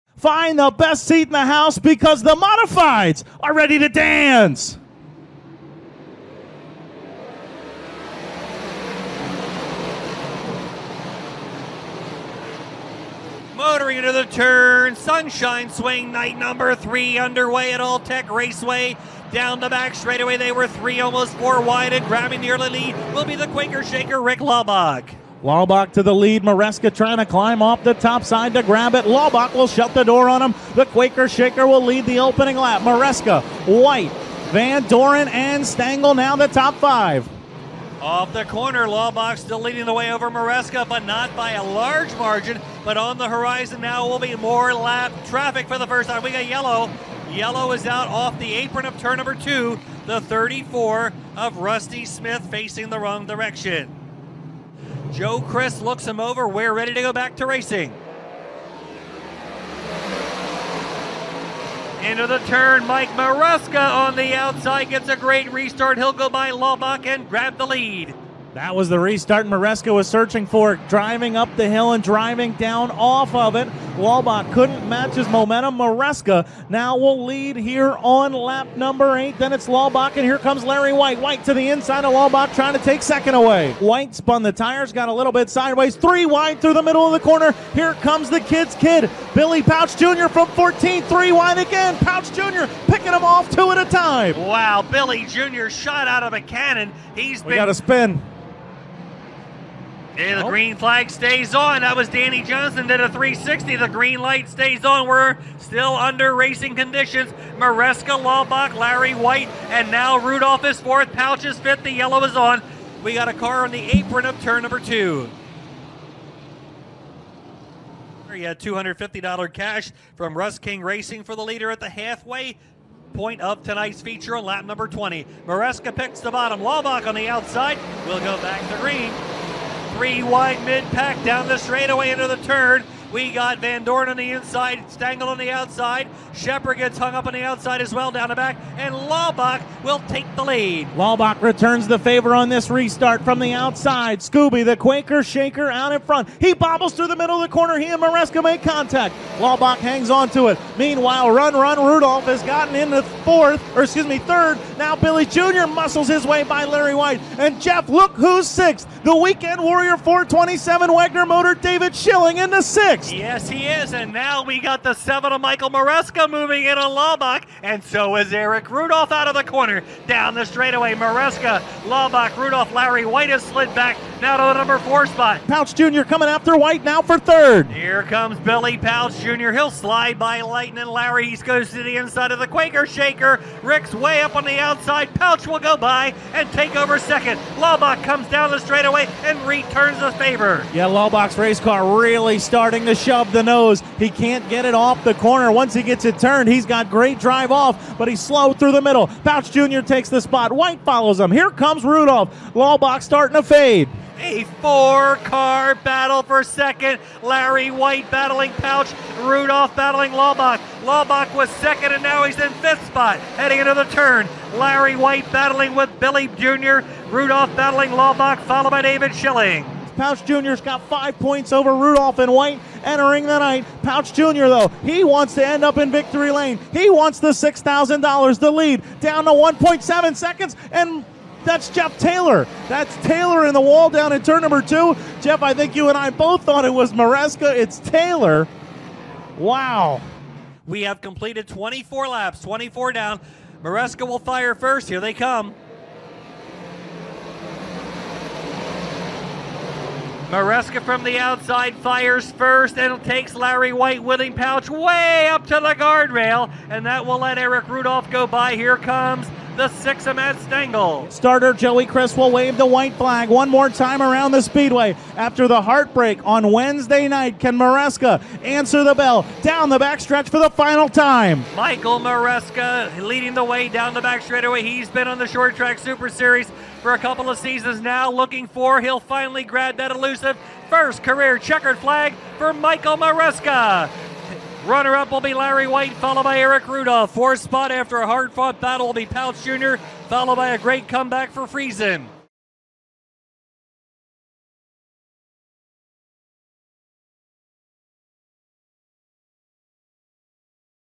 Short Track Super Series (2/11/22) at All-Tech Raceway.
Highlights from the Short Track Super Series Sunshine Swing event at All-Tech Raceway.